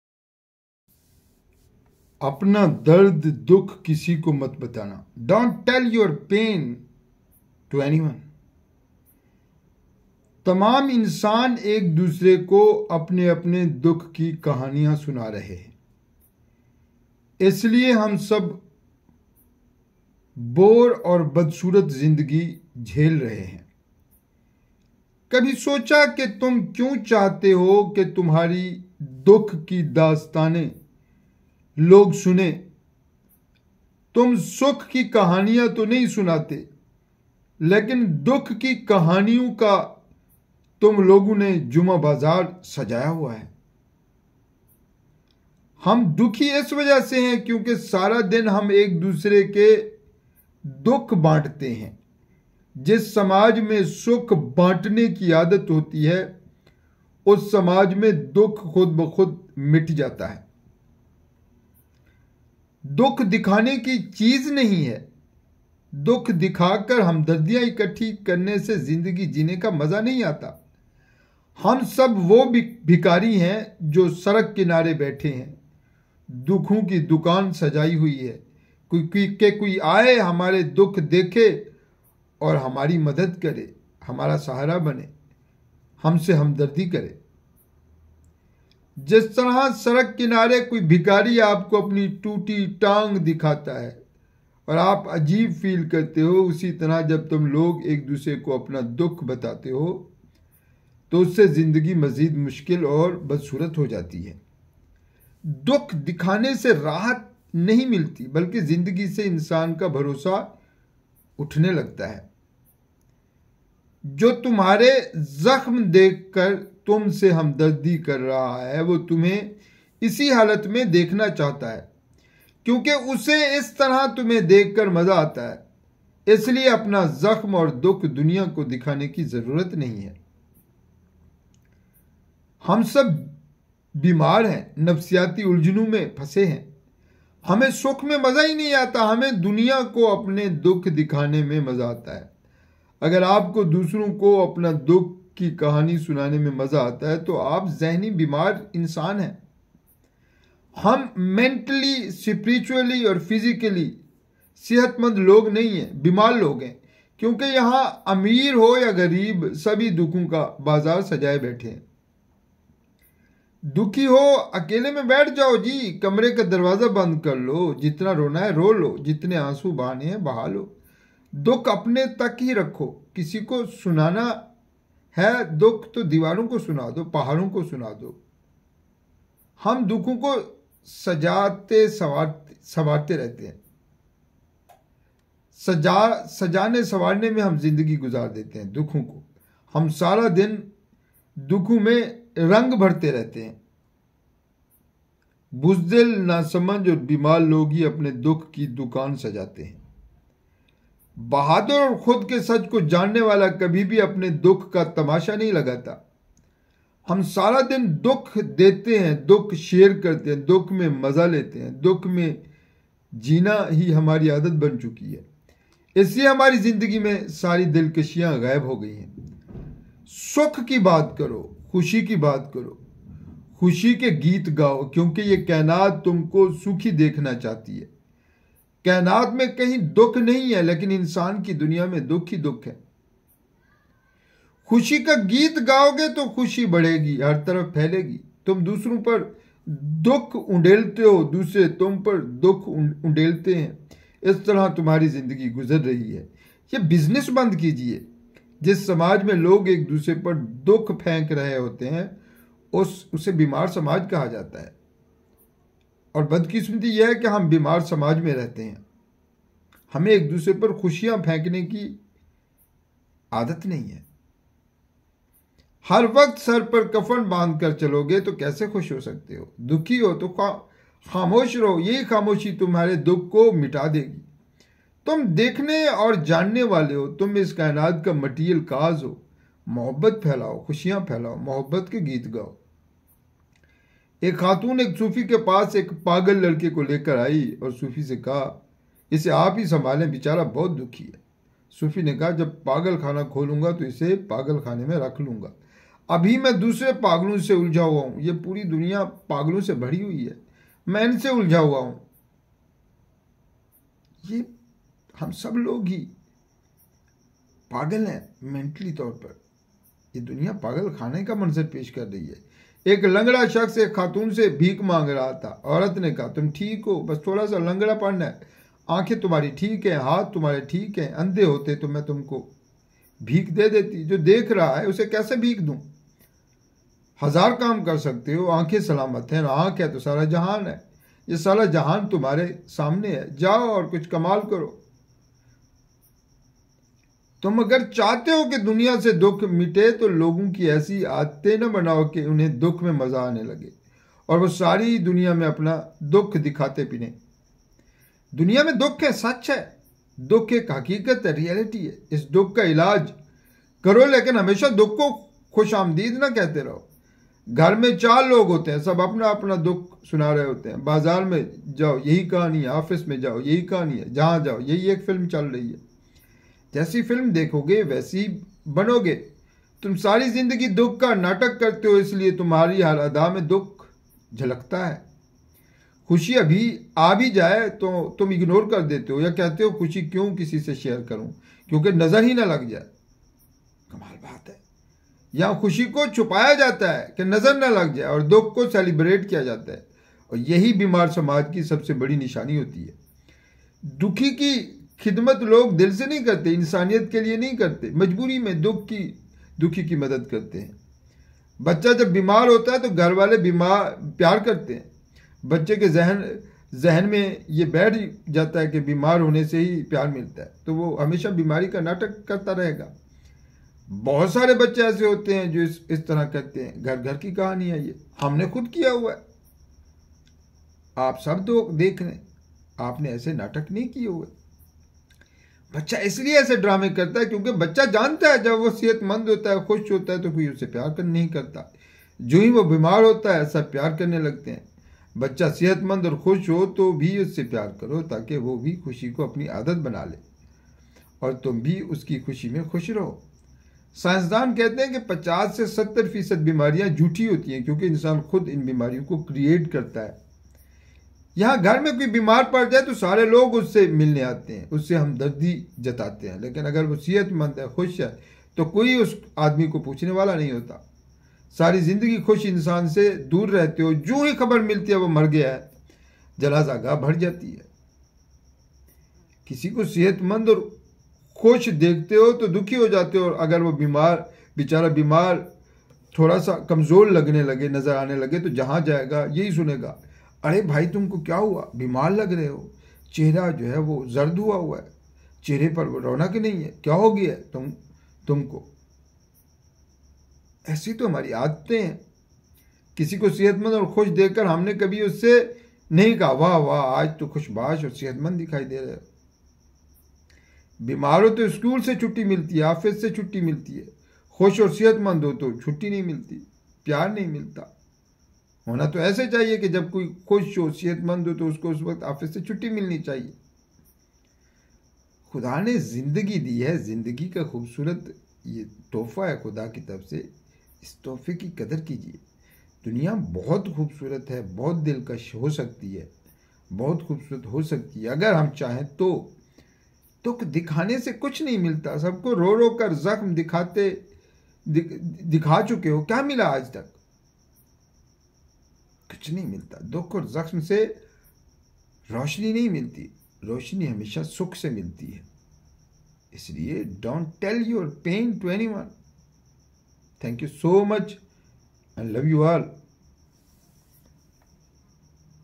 Happy Living Tips | Urdu | Hindi
In this video, we explore the culture of sharing sad stories that has become commonplace in society. We question why we are drawn to sharing our sorrows and how it affects our daily lives. We discuss the negative impact that this cycle has on our mental, spiritual, and physical health, and how it contributes to a sick society. Instead, we suggest that by spreading love and happiness, we can create a more positive environment. We offer tips on how to break the cycle of sharing sad stories and focus on the good in life. This video is for anyone who wants to live a happier life and be a positive influence on those around them. So, join us in spreading love and happiness, and let's break the cycle of sadness together.